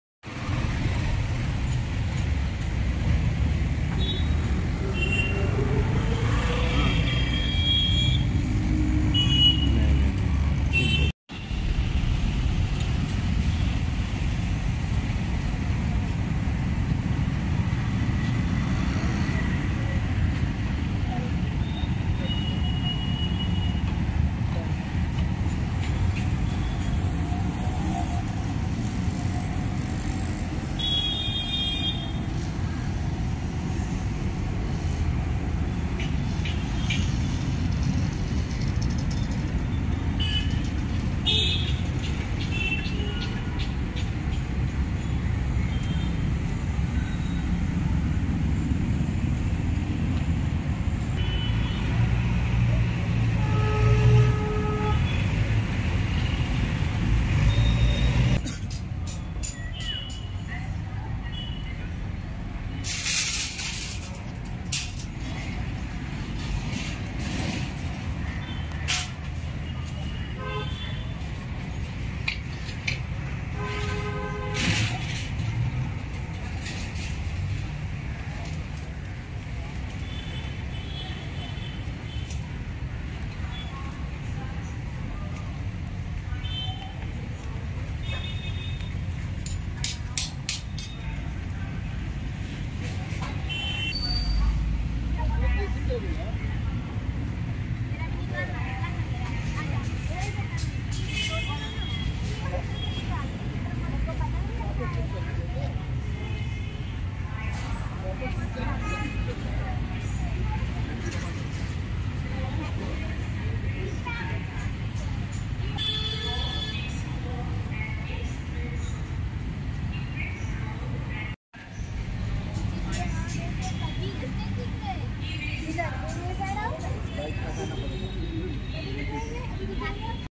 Beautiful Christmas scene outside ORLEM Church in Malad.
Christmas festival is going on everywhere in Mumbai cities. Beautiful Christmas scene outside ORLEM Church in Malad.
Christmas biggest celebrated in Bandra City in mumbai